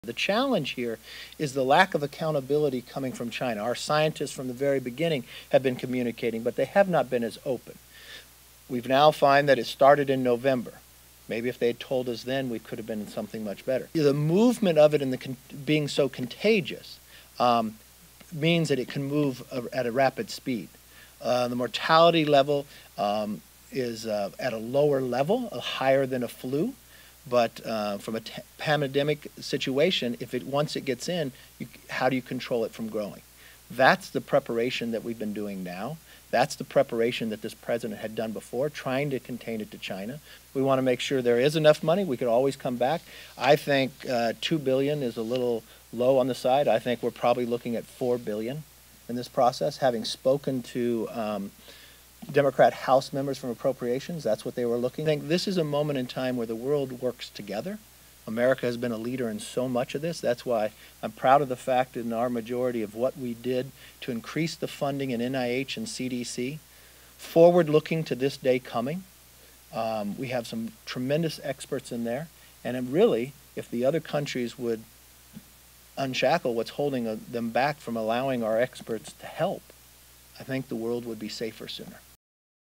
McCarthy: Virus challenge, China's accountability
House Minority Leader Rep. Kevin McCarthy says the challenge for the United States when it comes to coronavirus is "the lack of accountability coming from China." (Feb. 26)